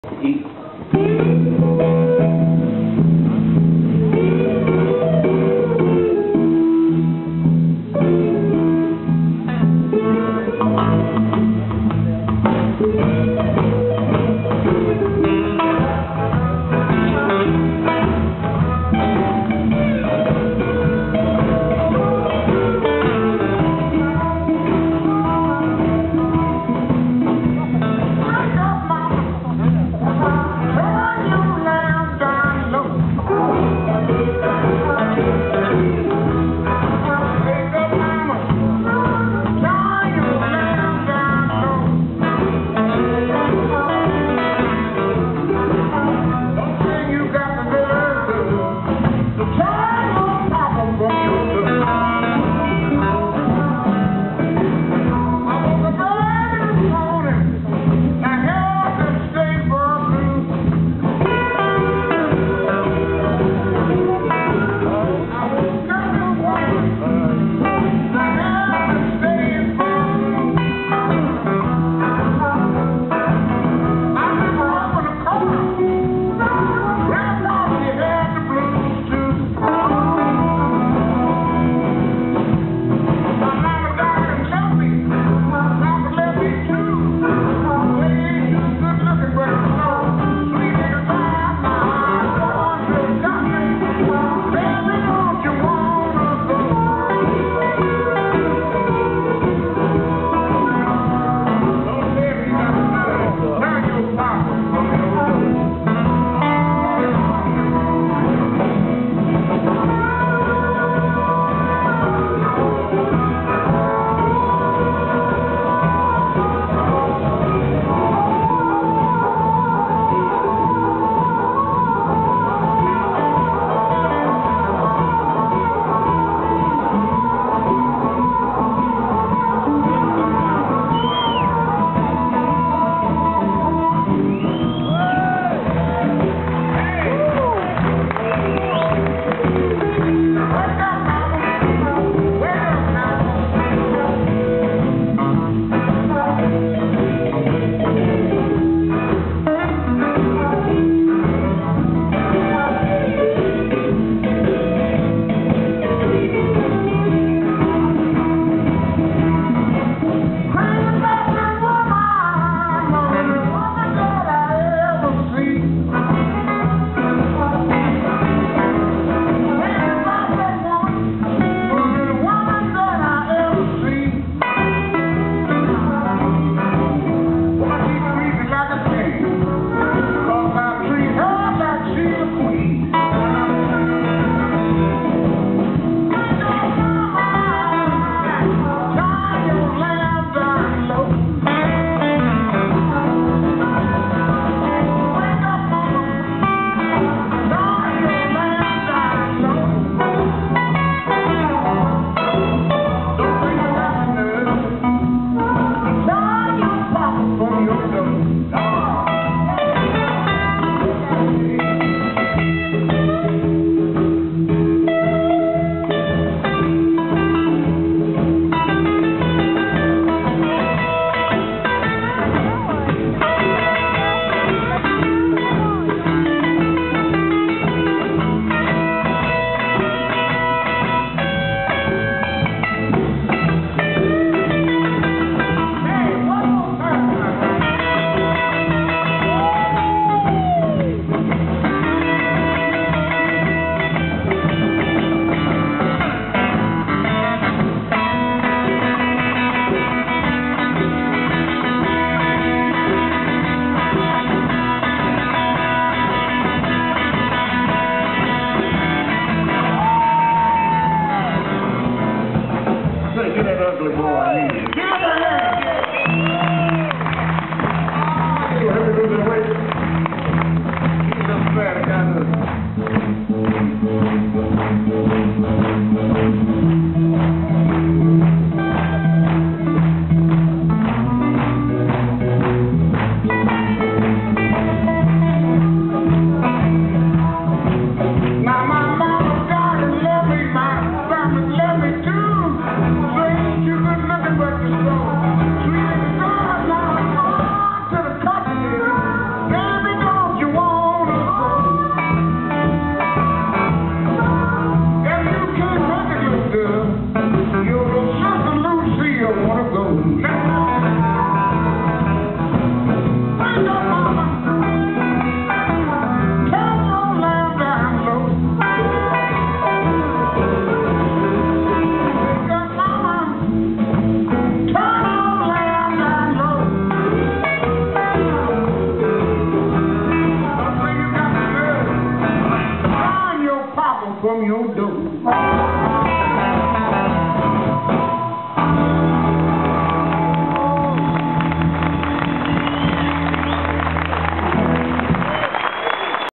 DAVE RILEY & TOMCAT COURTNEY & BOB CORRITORE live
TOMCAT COURTNEY & BOB CORRITORE & DAVE RILEY live à LA SEMAINE DU BLUES DE MONTFORT (ille et vilaine-35). Bob Corritore : harmonica - Tomcat Courtney : guitare/chant - Dave Riley : guitare- Jérome Boisneau (the cool drivers) : batterie - Dave Riley Jr : Basse